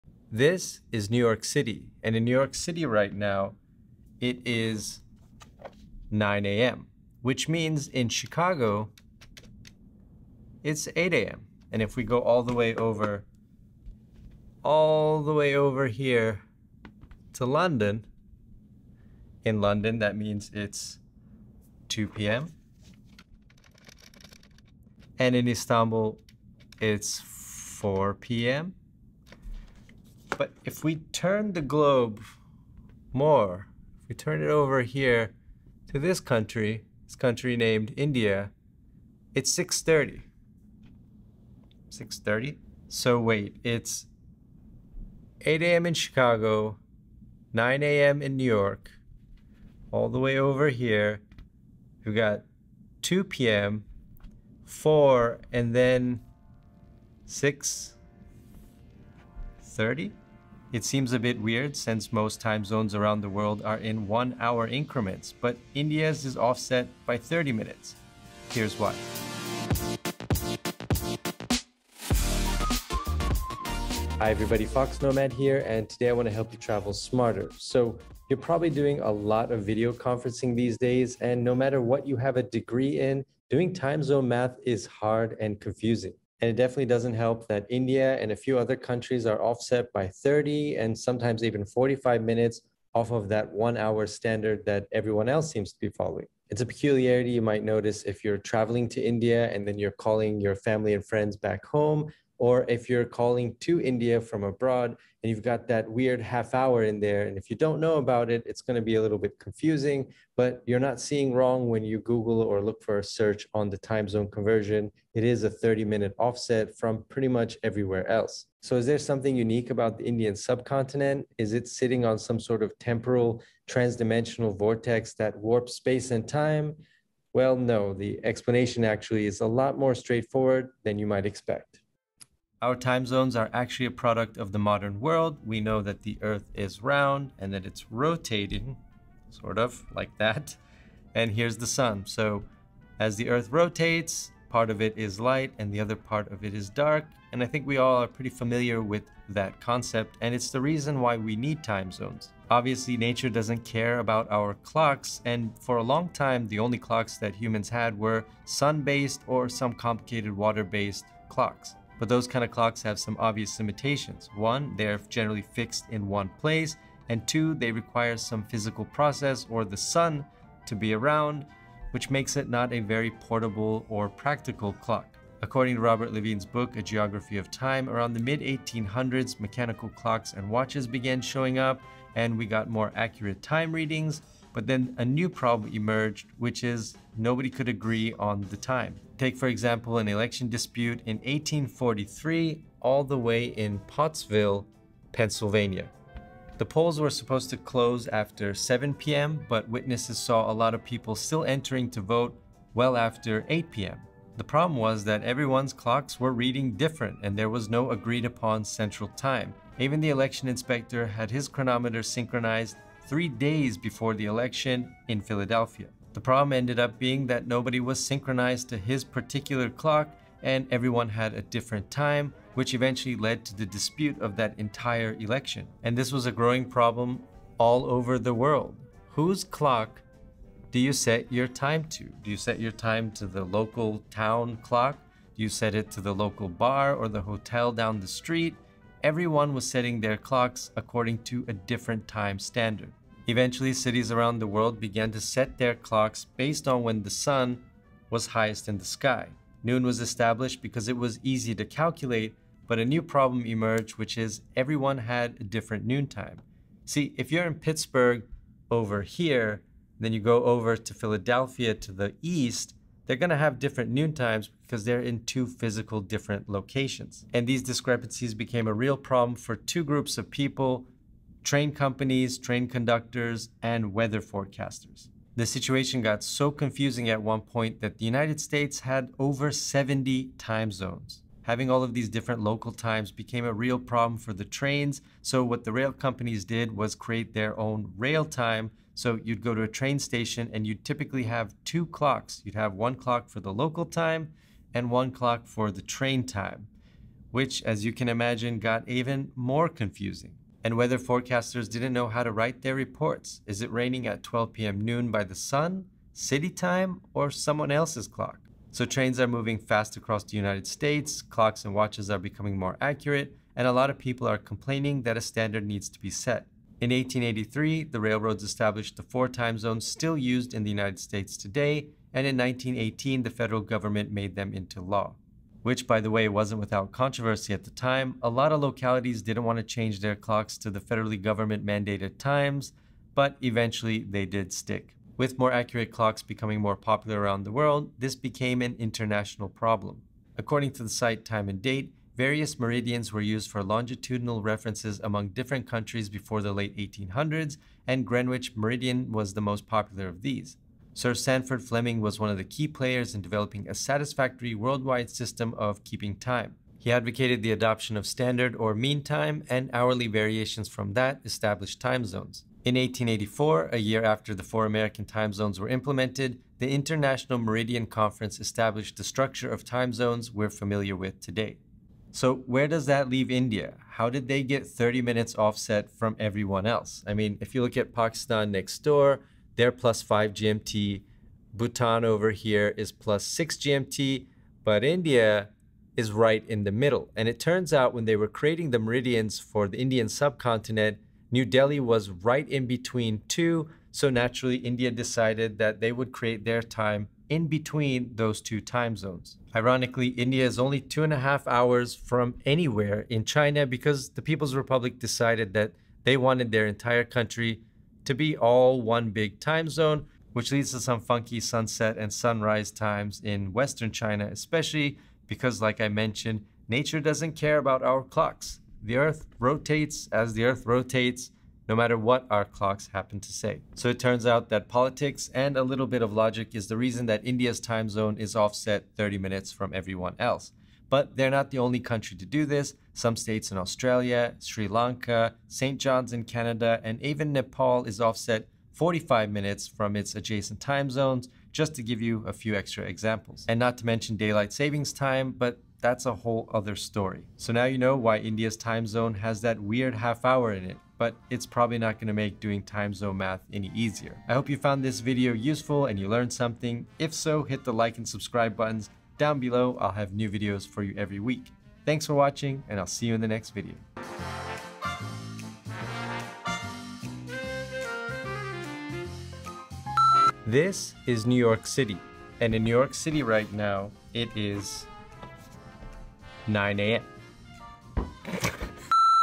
India's Time Zone Is 30 Minutes Off: Here's Why!
Time-space vortex? The explanation's a lot simpler than that.

A Geography of Time